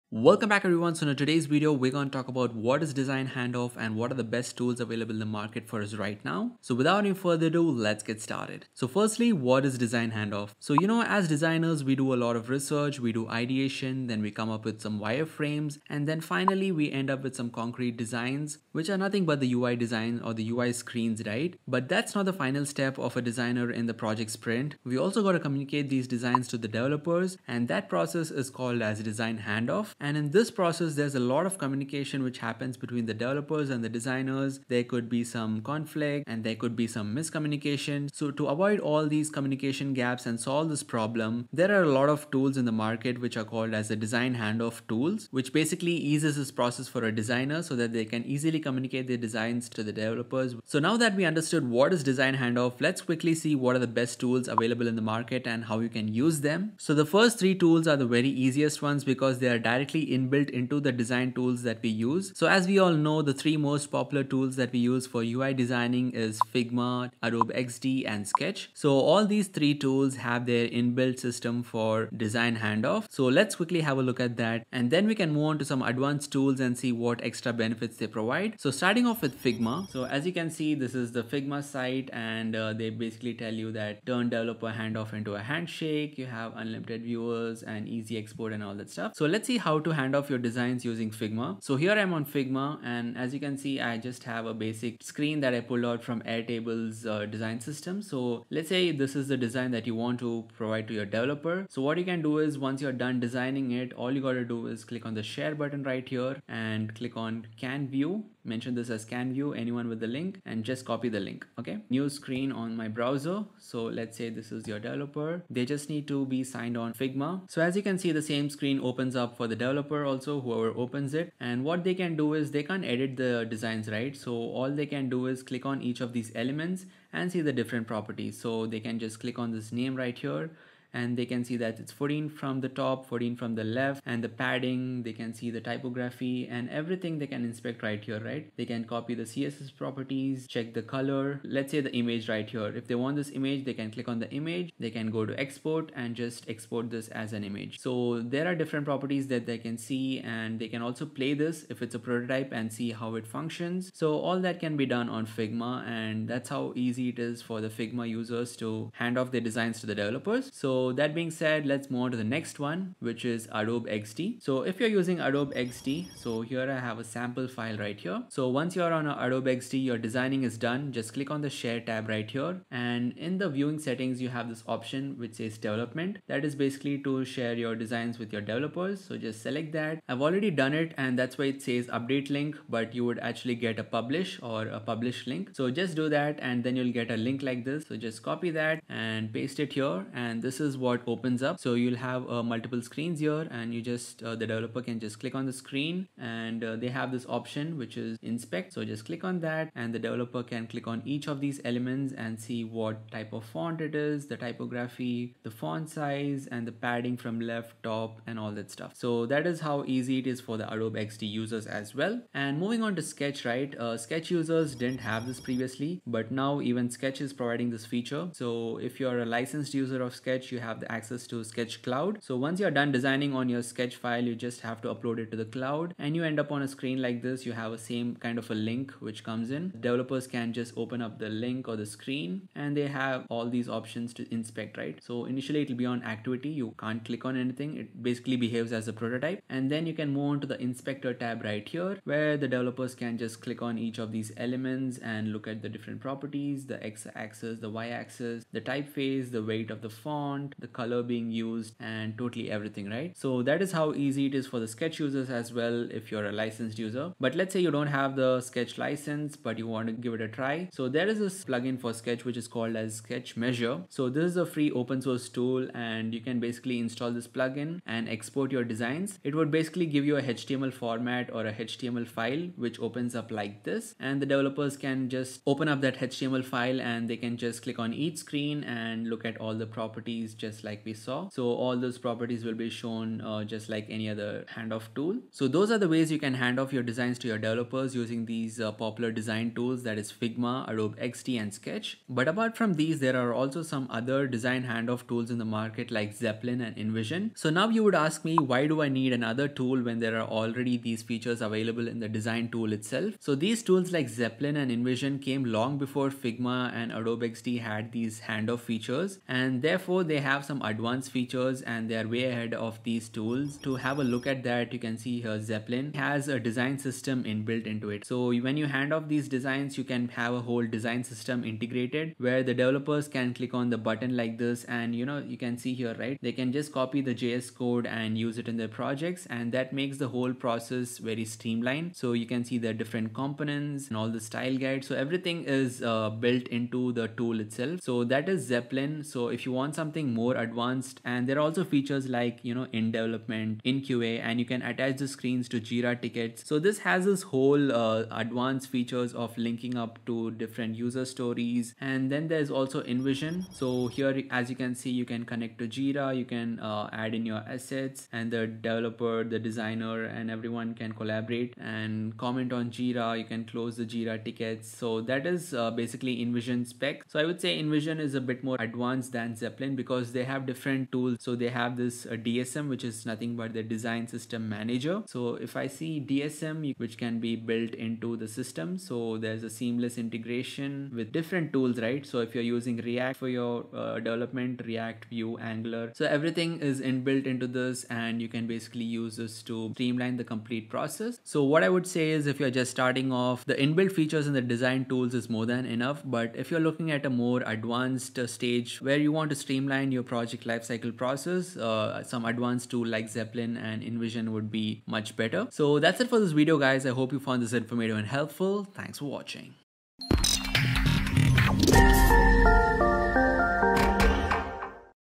What is Design Handoff | Best Design Handoff Tools?
In this video, I talk about what is design handoff, and the best design handoff tools available in the market Free & Paid. Basic Design Handoff tools that are built-in with the design tools like Figma, Adobe Xd & Sketch and then a couple of advanced Design Handoff Tools like Zeplin & InVision.

⏱️Timestamps:
0:00 - What is Design Handoff
0:54 - Best Design Handoff Tools
1:20 - Figma Design Handoff
2:47 - Adobe Xd Design Handoff
3:36 - Sketch Design Handoff
4:23 - Sketch Measure Plugin
5:14 - Zeplin
5:54 - InVision
6:32 - Final Words

Image & Animation Credits: InVision & Lottie.